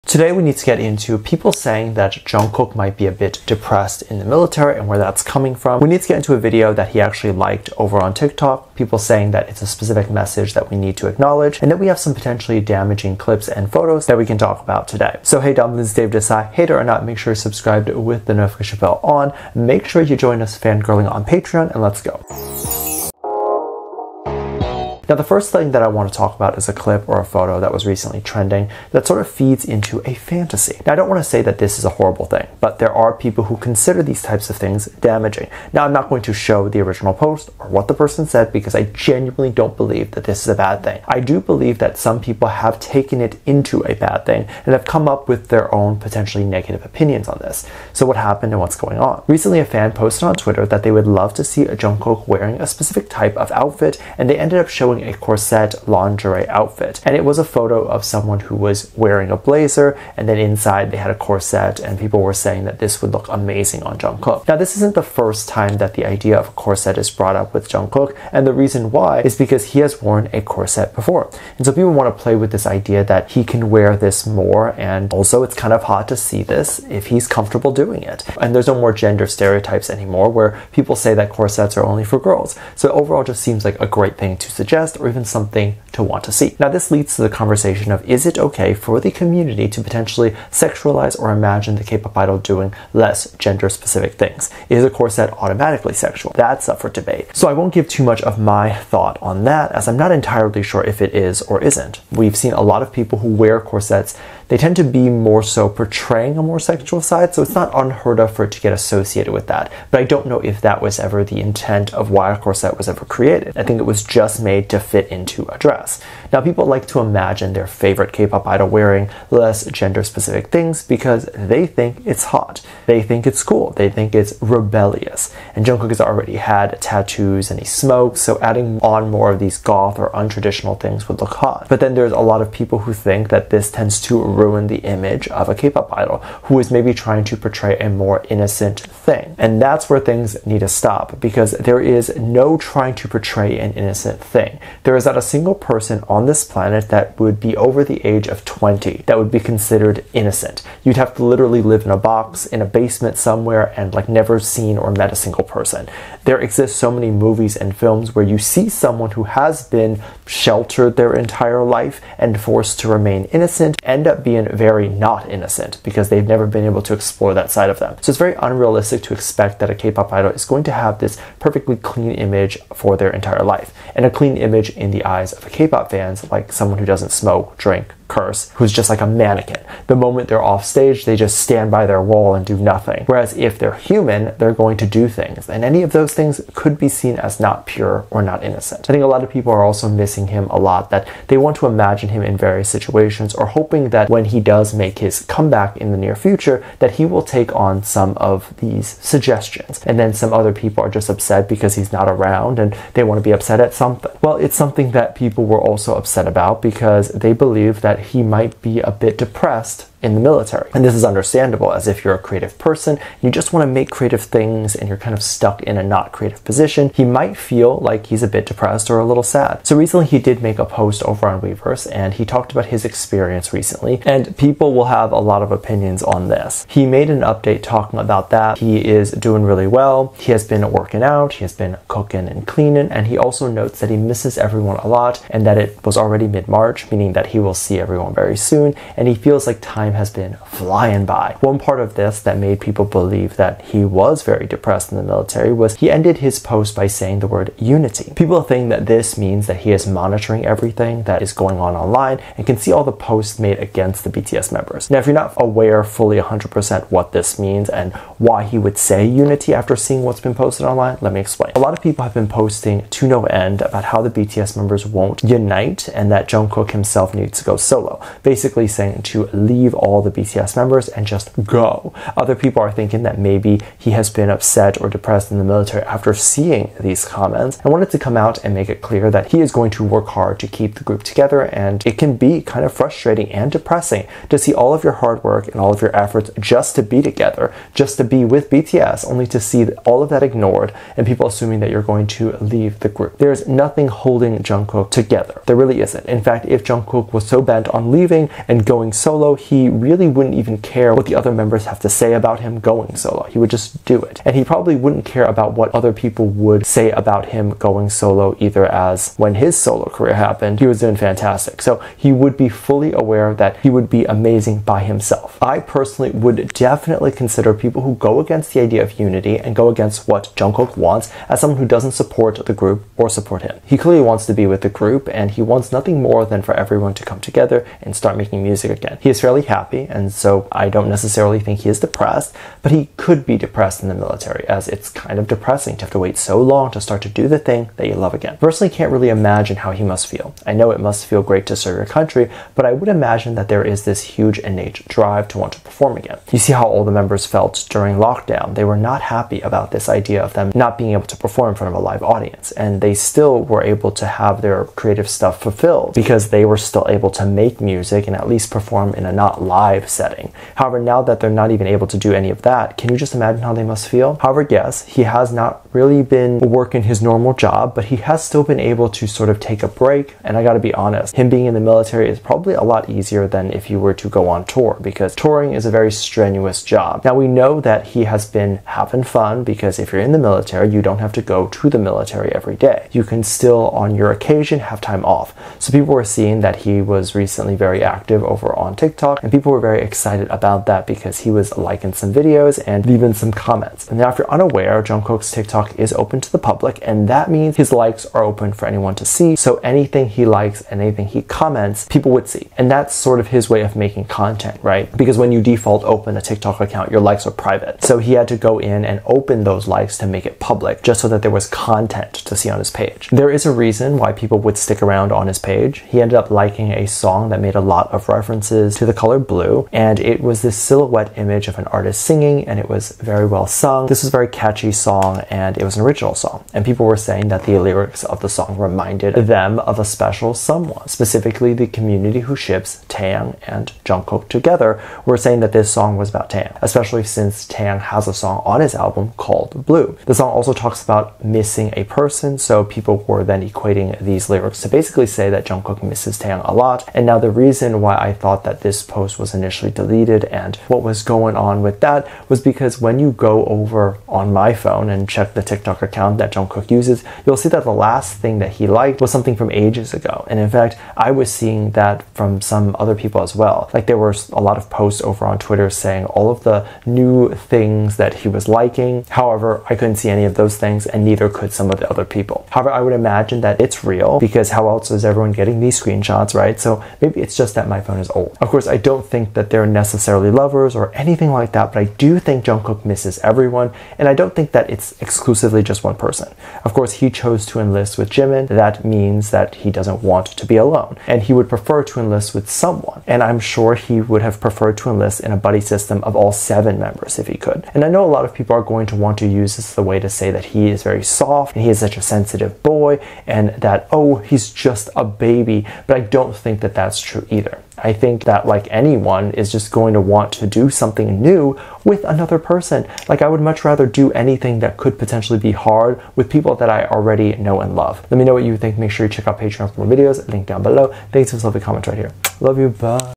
Military Reports JKs DEPRESSED! JK REMOVES NEW TIKTOK, Him Missing His LOVER?(rumor) BAD CLIP TRENDS
Hey Dumplings, today I want to get into more Kpop news specifically more BTS news. I want to get into potentially Jung Kook reporting that he is depressed. I want to get into Jung Kook's post not being visible and him rumored to be missing a lover. We also have a clip that people are saying might be damaging.

0:00 Table of Contents
0:25 Clip That Might Be Damaging
4:00 Is Jung Kook Depressed?
6:00 Jung Kook Missing His Lover?
8:38 What You Can Do Next To Support

Learn Korean!